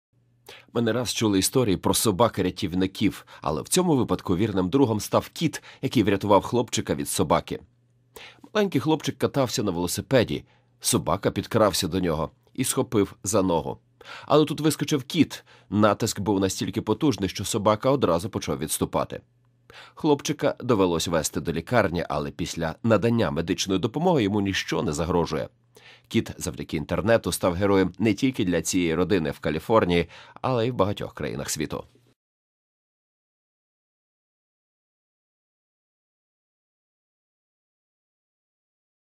Кіт врятував хлопчика від собаки
У Каліфорнії на допомогу хлопчику, якого собака схопив за ногу, кинувся кіт і хоробро відігнав нападника.